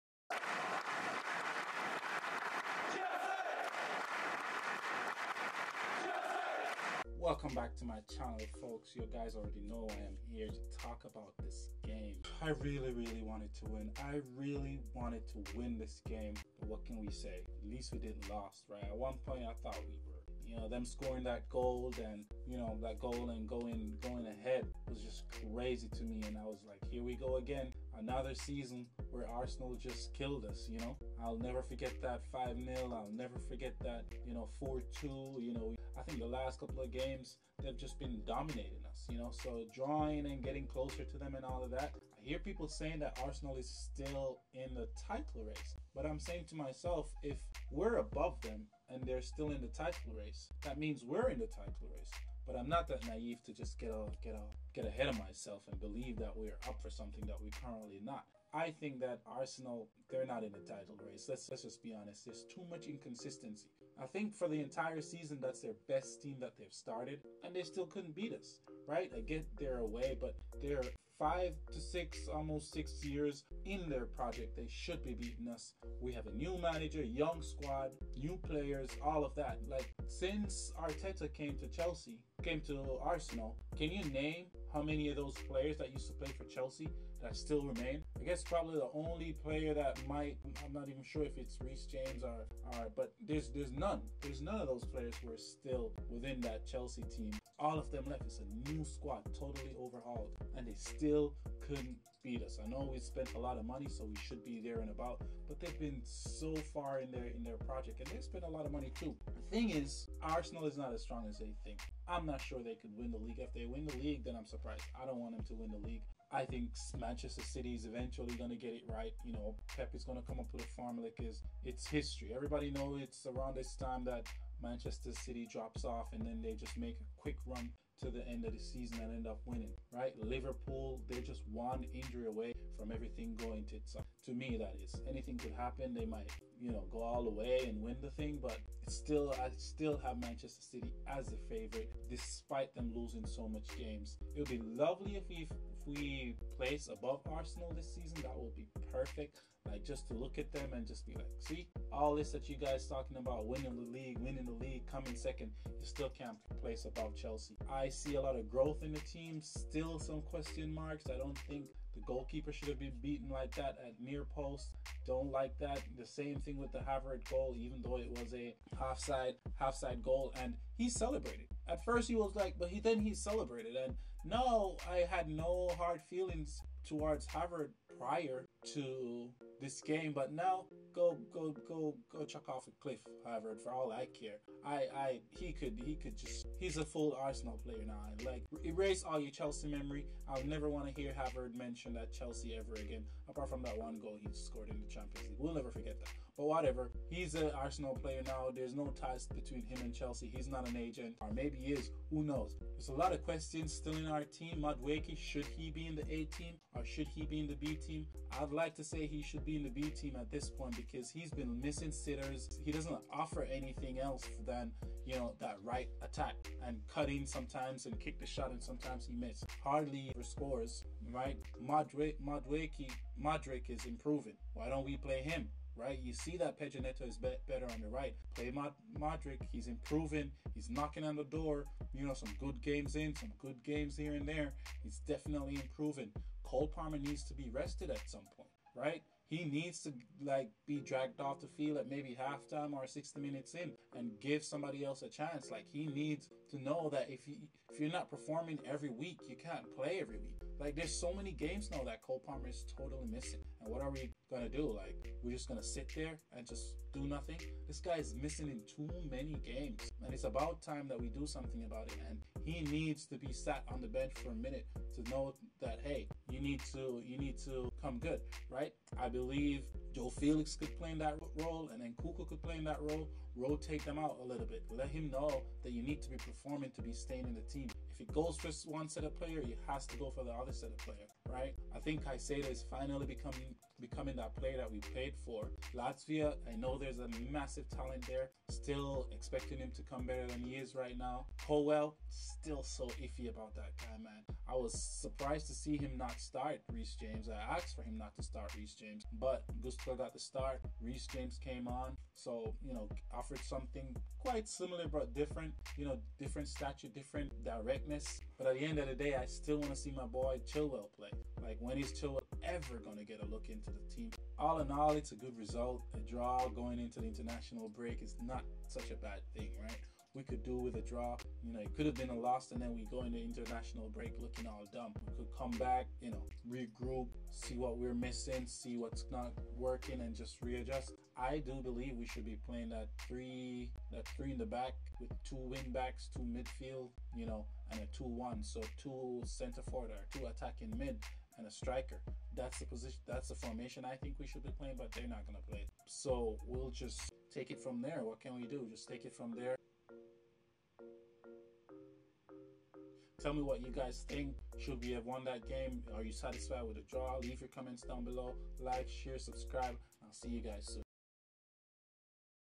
Intense Clash Ends in Draw! Chelsea 1-1 Arsenal | Frustration & Drama
In this video, I react to the intense showdown between Chelsea and Arsenal that ended in a dramatic 1-1 draw. From missed opportunities to moments of tension, this match had it all, and I’m breaking down the key highlights and controversies that kept fans on the edge of their seats. Watch as I share my frustrations, insights, and the emotions every fan felt in this hard-fought battle.